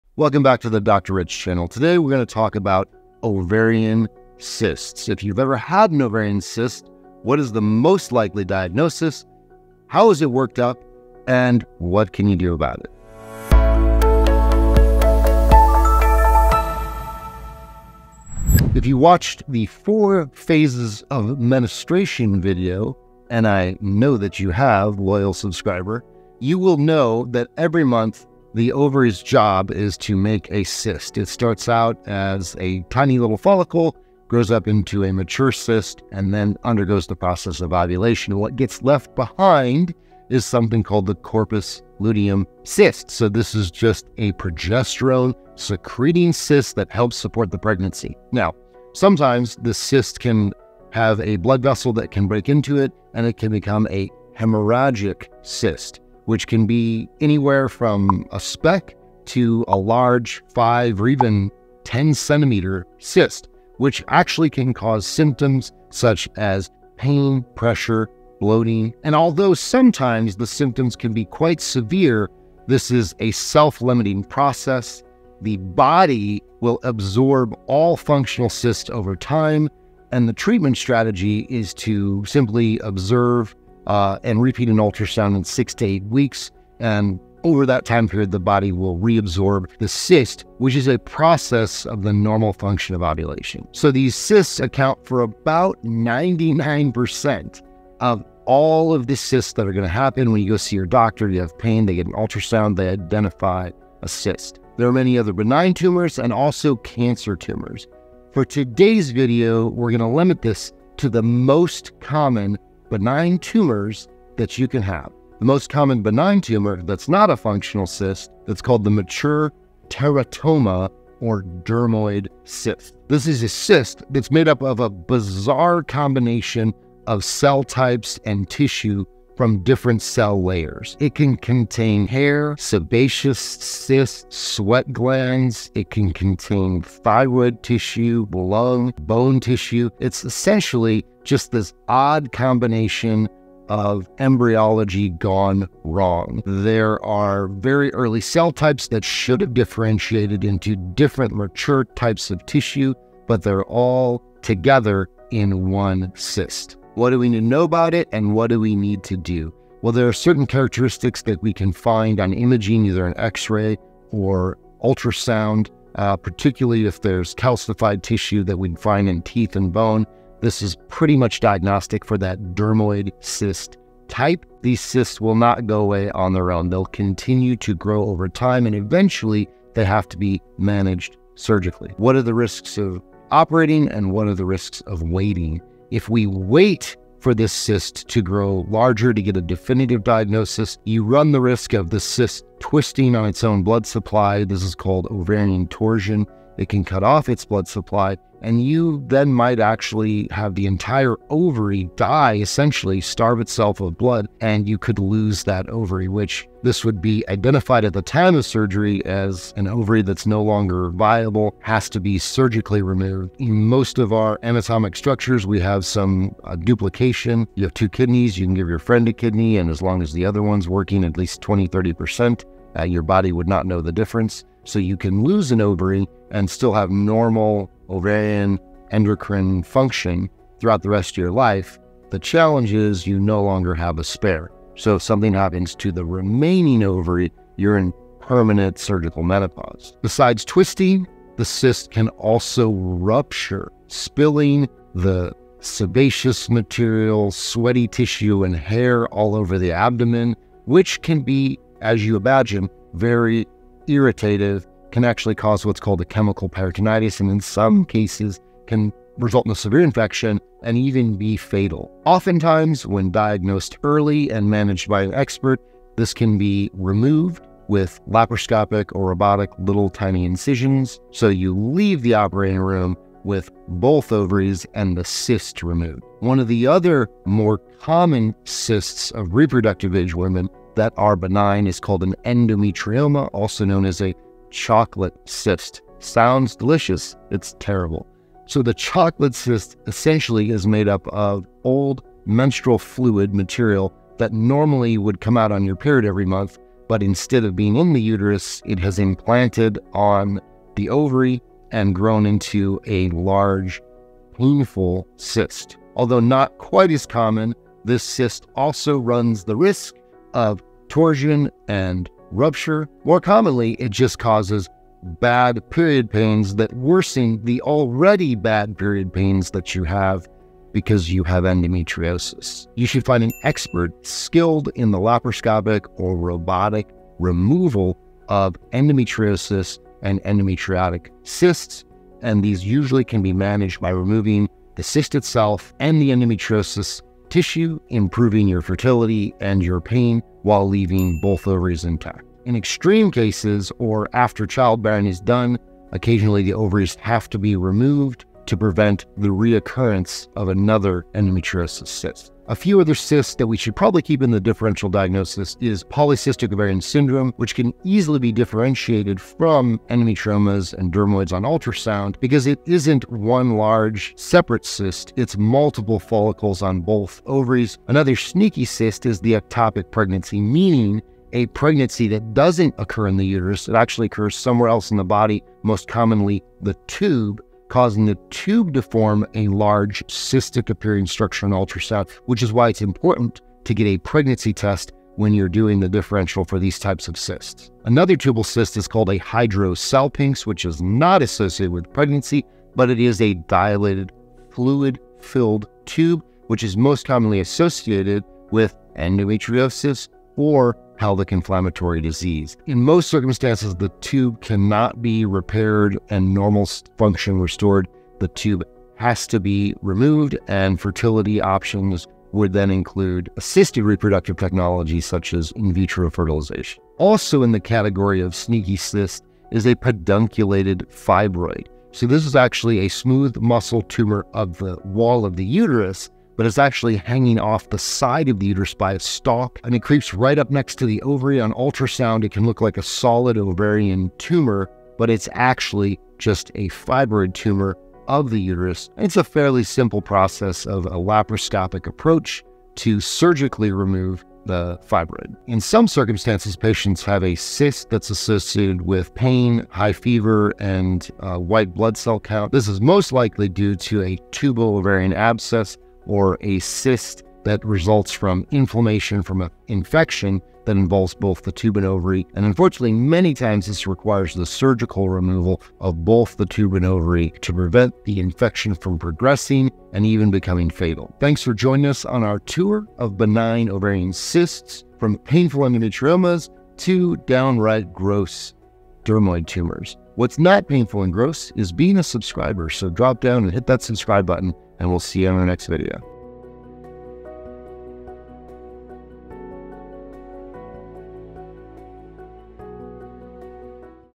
Ovarian Cysts Filled with Hair, and Teeth?!
🔴 What is your favorite content?
🔴 What could we do better?

There are many kinds of ovarian cysts.

Some are a normal part of the menstrual cycle. But today we’re talking about OTHER common cysts that can cause pain, pressure, bloating, and other issues.

These cysts can contain:
• blood,
• endometrial tissue,
• thyroid tissue, or even
• hair, teeth, and sweat glands!

In my new video, we’re talking about the most common benign ovarian cysts — what they are, how they’re diagnosed, and how they’re treated.